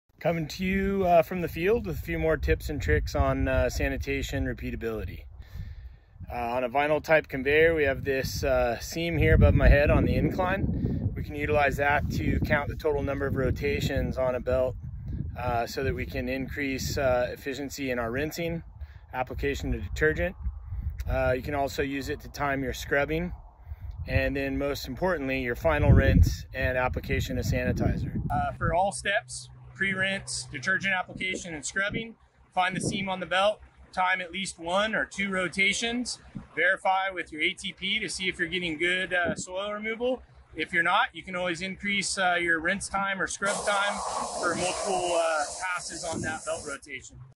Quick Tips for Field Sanitation
Using Belt Seams for Repeatability When Sanitizing Field Harvest Equipment​

When to use belt seams for repeatability:
- Pre-rinse​
- Detergent application​
- Scrubbing

How to use belt seams for repeatability:
- Find the seam on the belt​
- Sanitize for several rotations​
- Verify using ATP​
- Repeat if necessary